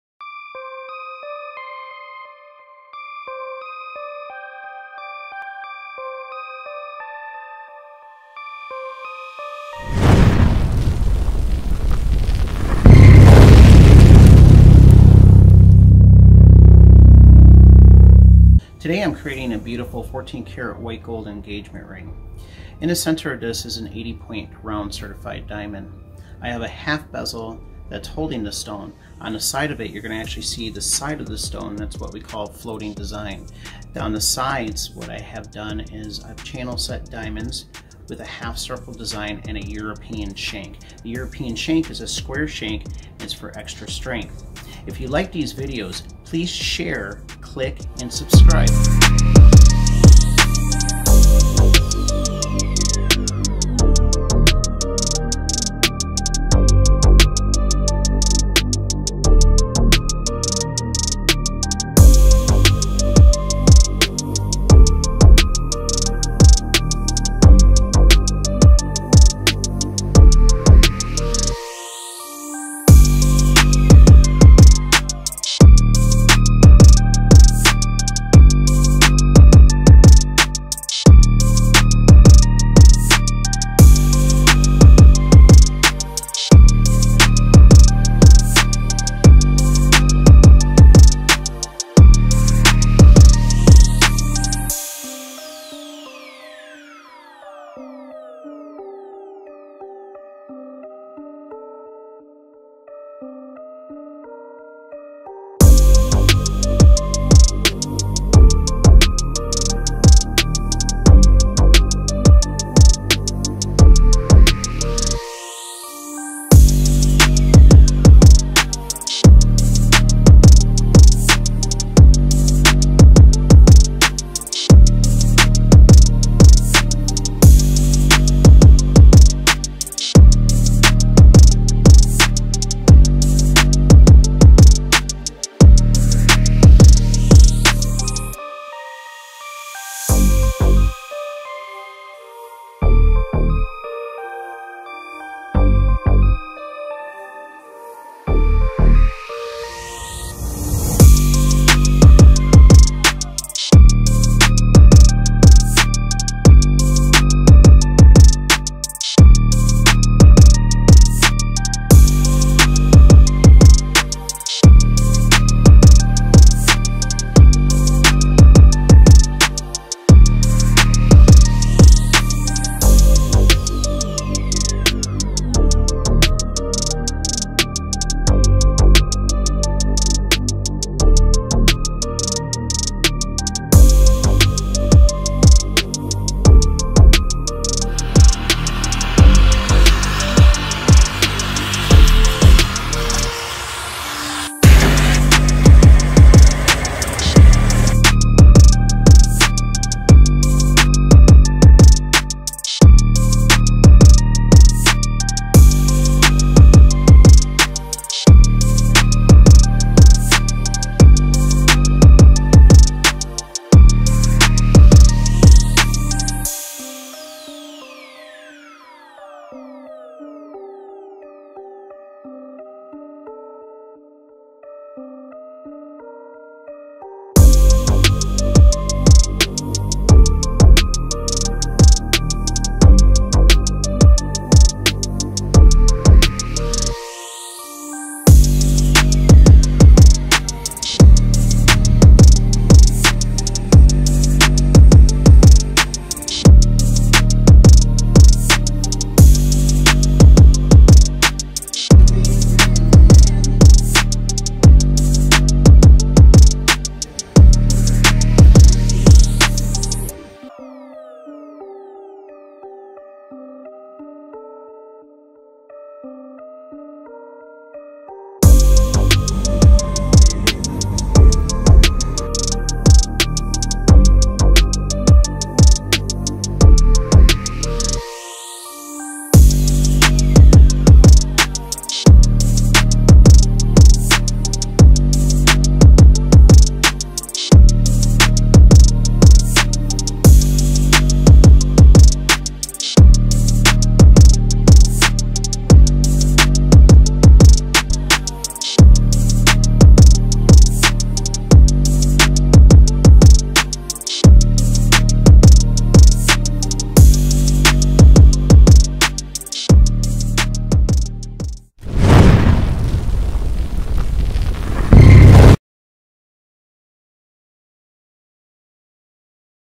How to Make a Custom Handmade Gold Engagement Ring
This ring is made out of 14k white gold engagement ring. In the center is a round diamond that is set in a half bezel. A round diamond melee is channel set down the shank.